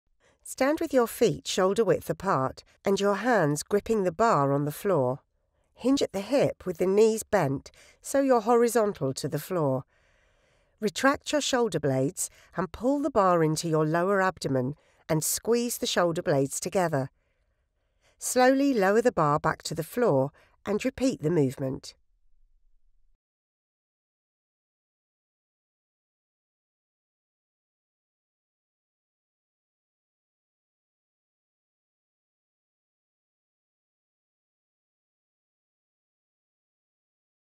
Pendulum Row | Upper Back | Strength and Conditioning Exercises
Stand with your feet shoulder width apart and your hands in a gripping the bar on the floor, hinge at the hip with the knees bent so you're horizontal to the floor. Retract your shoulder blades and pull the bar into your lower abdomen and squeeze the shoulder blades together, slowly lower the bar back to the floor and repeat the movement.